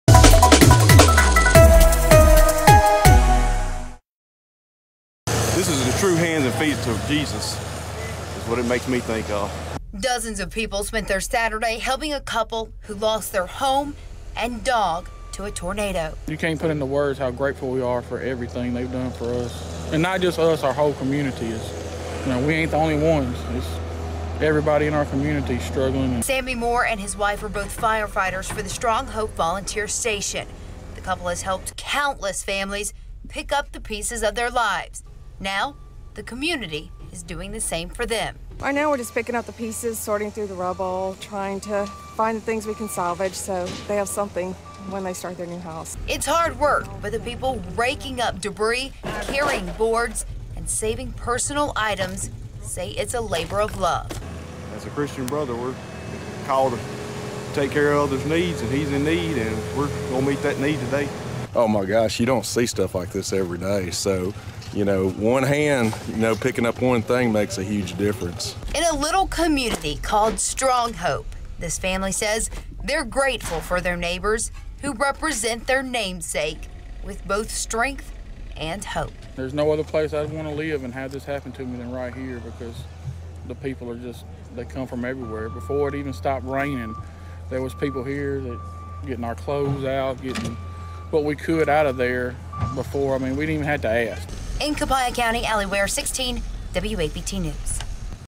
Volunteer firefighter gets help with clean up
A volunteer firefighter in Copiah County is getting a lot of help after losing his family home to a tornado earlier this week.